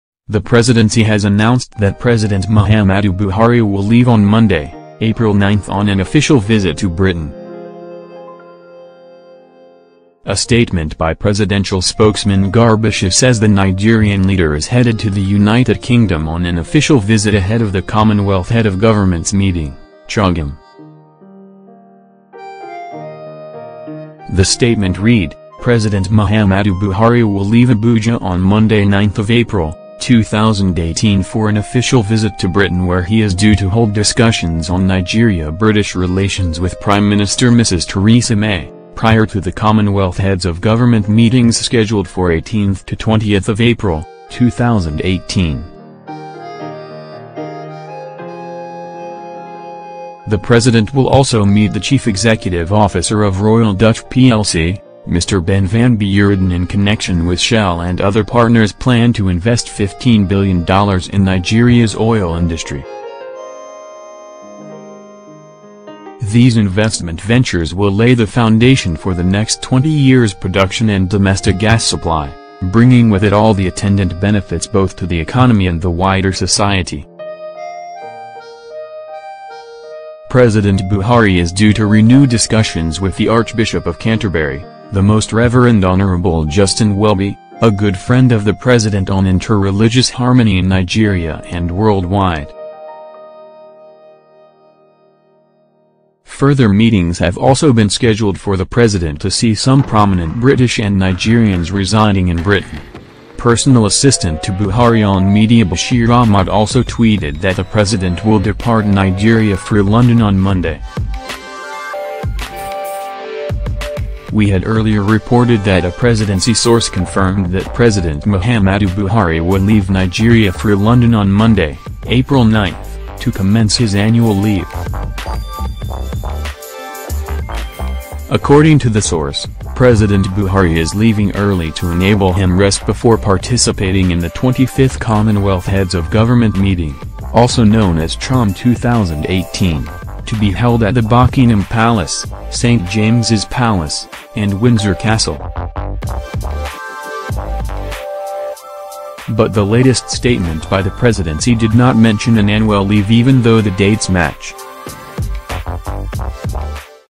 President Buhari to depart Nigeria for London on April 9
- A statement by the Presidency says Buhari will leave on an official visit to United Kingdom

- President Buhari is scheduled to meet with British Prime Minister Theresa May and other prominent people in that country

- The Presidency also confirmed that Buhari will take part in CHOGOM between Wednesday, April 18 and Friday, April 20